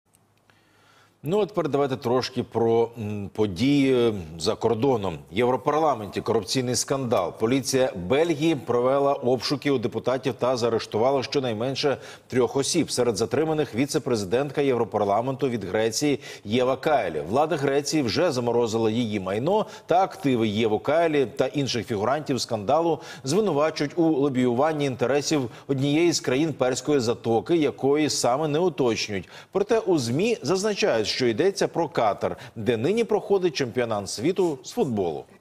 Віцепрезидентку Європарламенту затримано за підозрою у корупції
Віцепрезидентку Європарламенту Єву Кайлі та ще трьох людей було звинувачено та взято під варту в Бельгії у справі про корупційні зв’язки з Катаром.